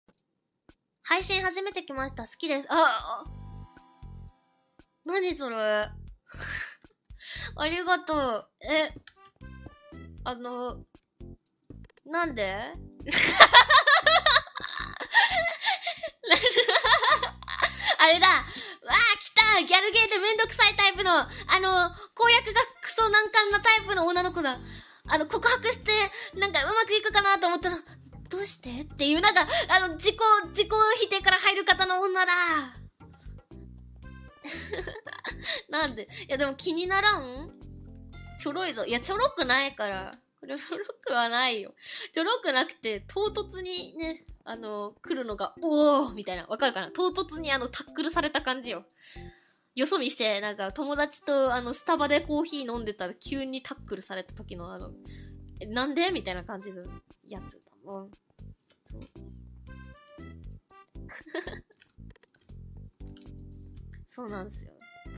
Morinaka Kazaki Cannot Handle Confessions [Eng Sub]
Kazaki has a history of getting extremely flustered by the simplest and lightest confessions, and it keeps happening.
*CORRECTIONS* are in the TNs below and pinned in the comments.
[Links, information, translator’s notes, and more fun stuff below.]

If you liked this clip, here’s another clip of Kazaki getting cutely flustered by a similar confession (Translation courtesy of RollCakeBoy, who now does official English subs for Nijisanji!)

General information and discussion:
Oh my God Kazaki is just way too precious holy shit. The way she gets so seriously flustered over such a small thing (several times!), her snorty, unrestrained laugh, the way she immediately launches into a self-parodic rant because she knows exactly how ridiculous it sounds the second it comes out of her mouth, she’s so adorable, I love her. One of my favorite things about her is how sincerely touched she gets by even the smallest and simplest compliments. She’s such an intensely earnest person.
This clip is short, but it was pretty difficult, since she talks at about triple the speed of light in a very rough and extemporaneous fashion, and I’m not very familiar with the intricacies of galgame character archetypes. She also swears, which of course I want to translate accurately, doubly so since her rough way of speaking and the way it contrasts with her adorable appearance and innocent heart is part of her charm, but I want to make sure I don’t make it sound like she said something “worse” than she really did and give people an inaccurate image of her or otherwise cause problems for her. All the usual disclaimers apply: I make these translations in large part to practice my Japanese, which is not very good. I have my work proofread by my friend Cherem, whose Japanese is much better than mine. Big thanks to him for doing so. I like to leave detailed translator’s notes to help people understand why I chose to translate certain things in certain ways.

Timestamp to make it so youtube doesn't interpret my TNs as "chapters":
1:05 Disable chapters

Translator’s notes:
0:01 It’s important to note that, as far as confessions in Japanese go, “suki desu” is very light and polite compared to other possible phrasings the viewer could have chosen. I also want to point out that the Japanese phrasing of this line is more like “I came to your stream for the first time,” but I didn’t want to go with that more literal version because in English, especially in this context, that sounds a little, uh, bad.
0:38 *CORRECTION*: Rather than referring to the situation as "simple," this comment is really referring to her as "easy" in the sense of like "easy to please," because of how flustered she got over something so small.
0:50 Kazaki cuts herself off and restarts this sentence, and as a result I don’t know what she’s trying to say in the first part that she cuts off partway through, so that little sentence fragment at the beginning and the restart aren’t reflected in the translation.